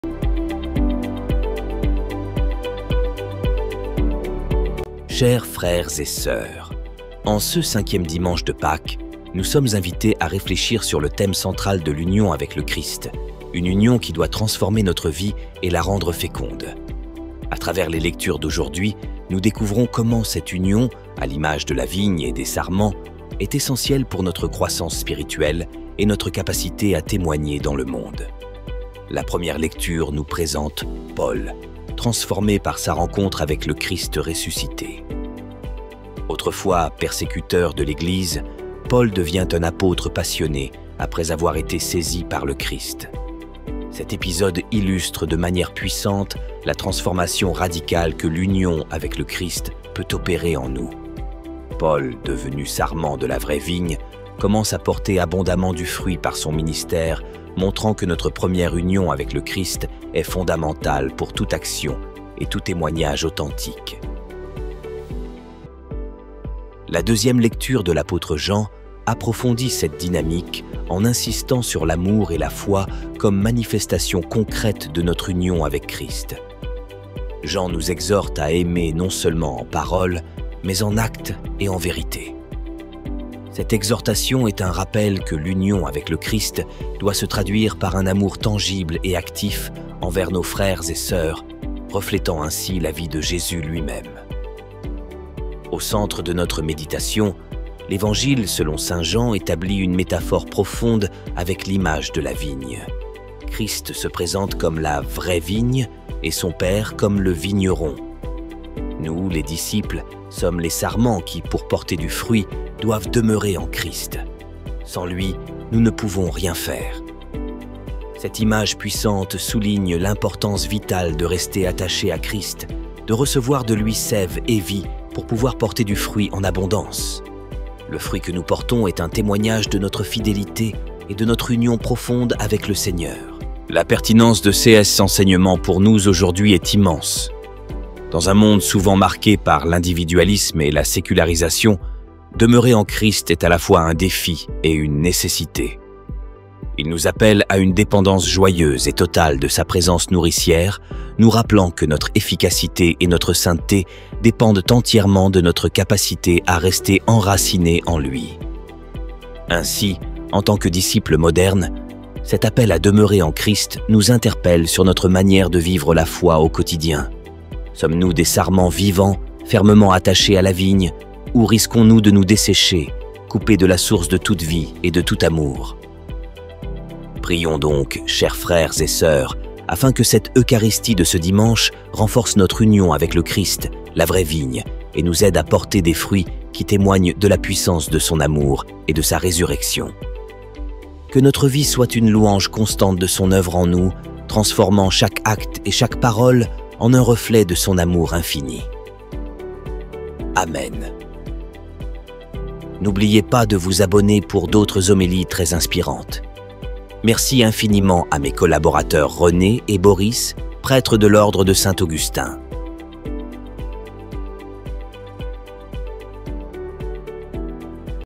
Homélie du 5e Dimanche de Pâques. Jean 15,1-8
Découvrez notre réflexion pour le 5e Dimanche de Pâques centrée sur Jean 15,1-8, où Jésus se présente comme la vraie vigne. Cette homélie explore l'importance vitale de demeurer en Christ pour porter du fruit dans nos vies. À travers les textes de ce dimanche, nous sommes invités à renforcer notre union avec le Christ pour une fécondité spirituelle et une transformation profonde.
Homélie, 5e Dimanche de Pâques, Jean 15, Demeurer en Christ, Spiritualité chrétienne, Vie de foi, Église catholique, Enseignement biblique, Évangile selon Jean